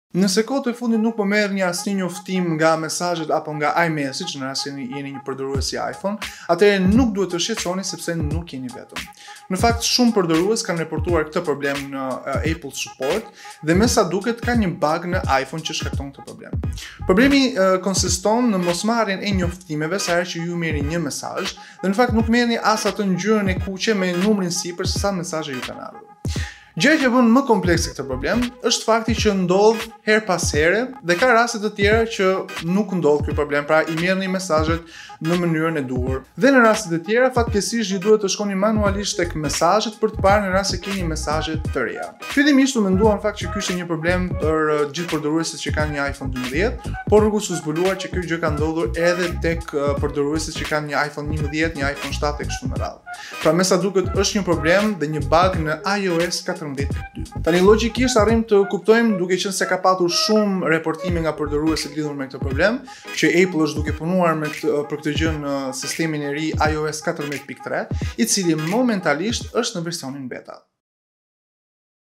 Një bug në iPhone nuk të njofton për mesazhet - Geek Room
Kur do kemi një ios14 të saktë? As ne nuk e dimë... Problemi i rradhës lidhet me njoftimet të cilat nuk po shkojnë në disa iPhone. Për ta zgjidhur duhet të kërkosh manualisht.

Host: Xhonald Mamo
Producer: Brenton Bënja